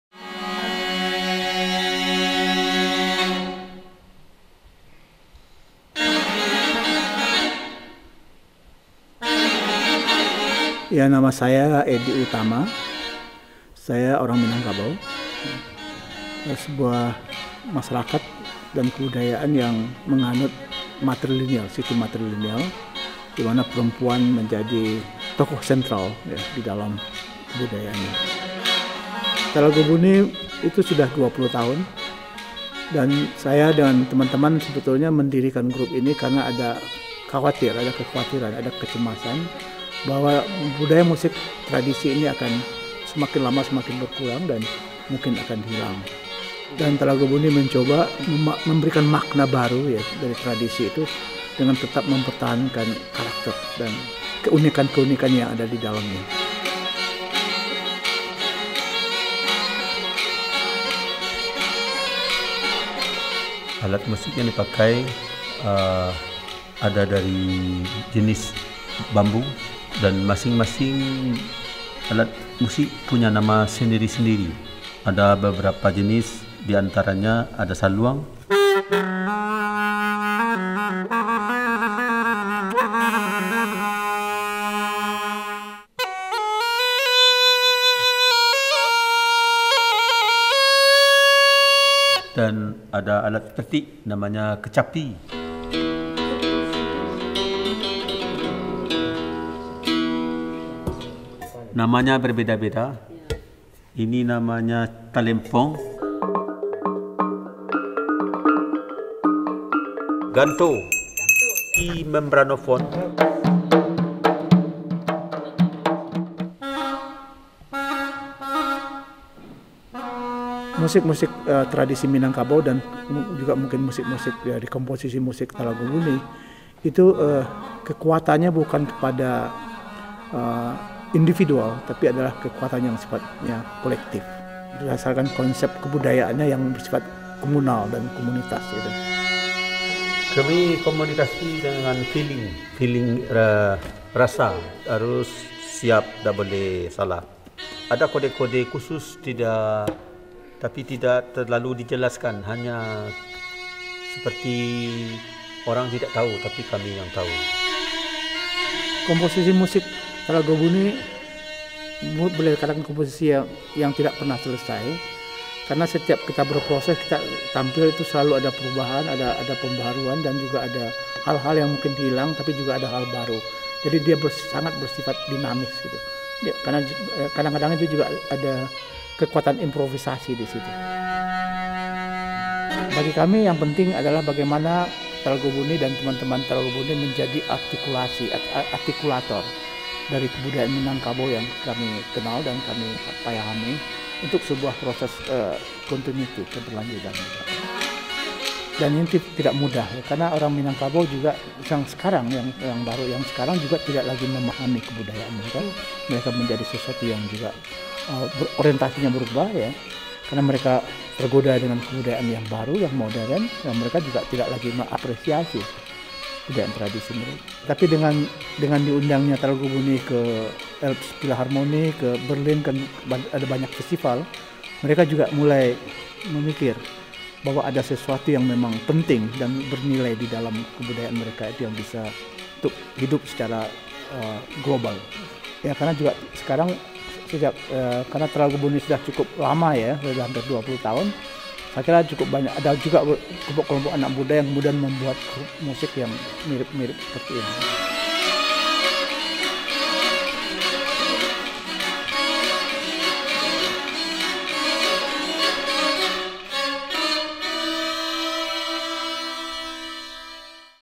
Elbphilharmonie | Talago Buni - Sumatras spirituelle Musiktradition
Was die Reihe »Klassik der Welt« im Kern bedeutet hat auch die Gruppe Talago Buni sich zur Aufgabe gemacht – Sie erhalten ihr musikalisches Kulturgut, indem sie es für uns sicht- und hörbar machen!